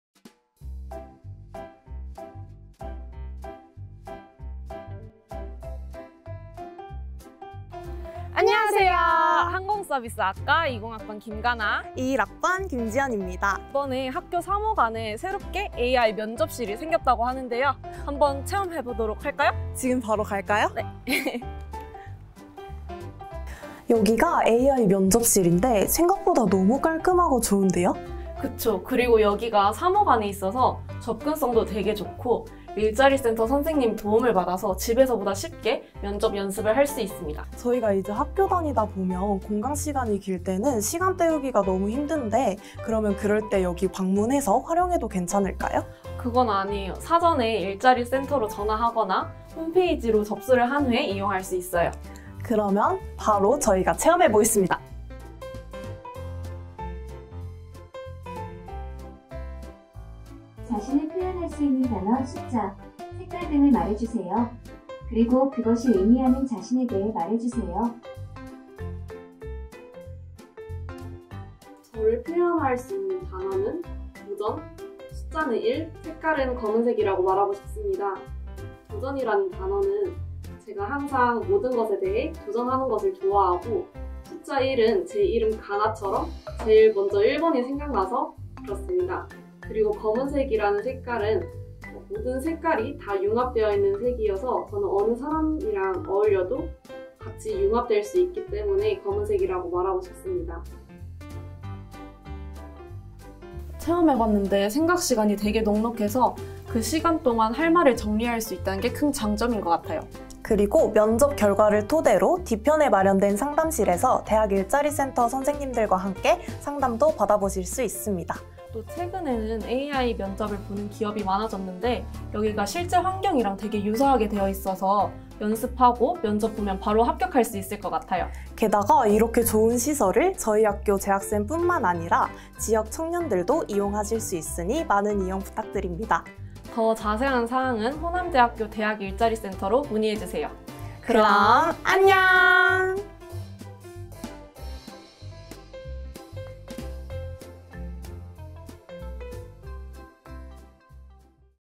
AI면접실 무료로 이용하세요! / 호남대 AI면접실 /외부인도 사용가능!
취업역량강화를 위한 AI면접실이 새로 생겼습니다!!!
실제 기업 면접상황과 유사한 환경을 조성했으니 많은 이용바랍니다!

★장소 :  상지관(3호관) 1층 대학일자리플러스센터
★신청방법 :

📌관련영상!!
AI면접 무료체험 방법과 진행내용 알려드립니다!
AI자기소개서 / AI화상면접 / AI역량검사